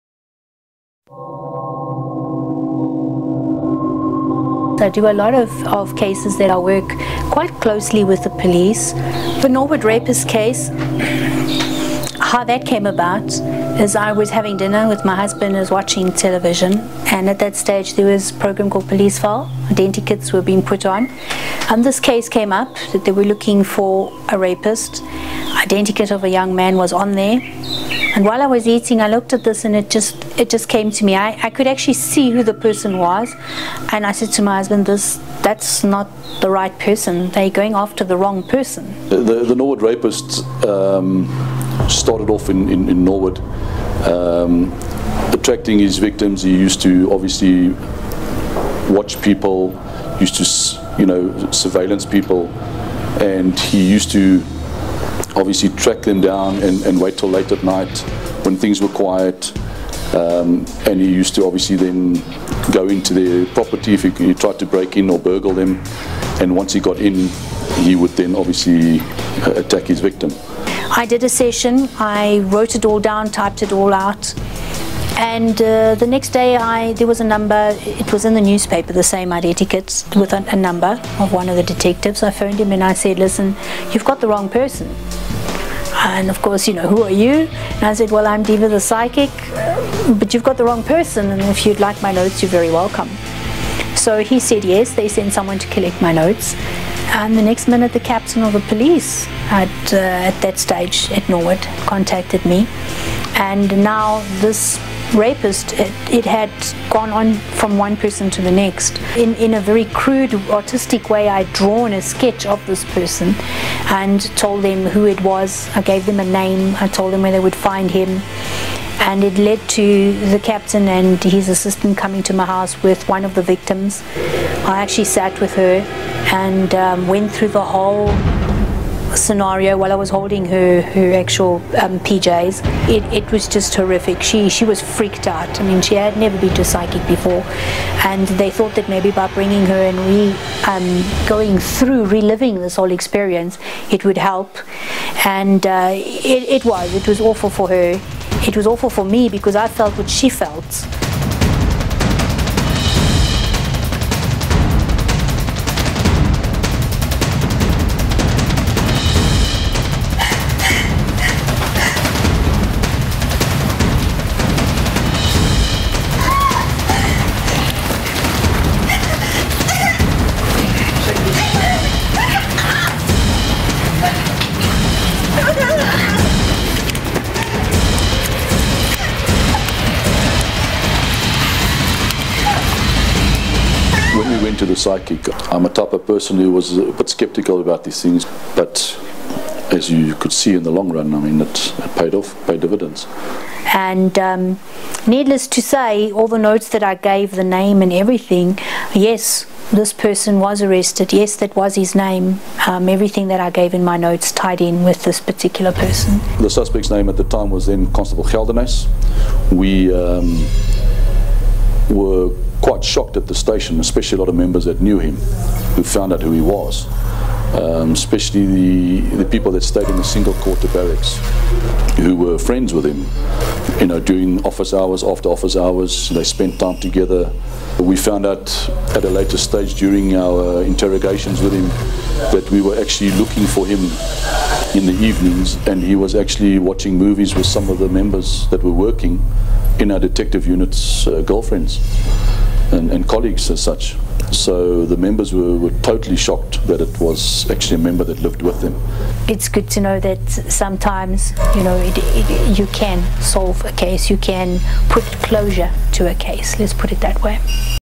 Diva the Psychic VERSUS Norwood Rapist serial killer - not for sensitive viewers!
A very scary and disturbing re -enactment of the Norwood Rapist serial killer case. Diva the psychic provided valuale clues and an identikit which proved 100% accurate down to the culprits name. Makes you wonder who can you actually trust around you. It is believed there were 8 victims, only 1 survived.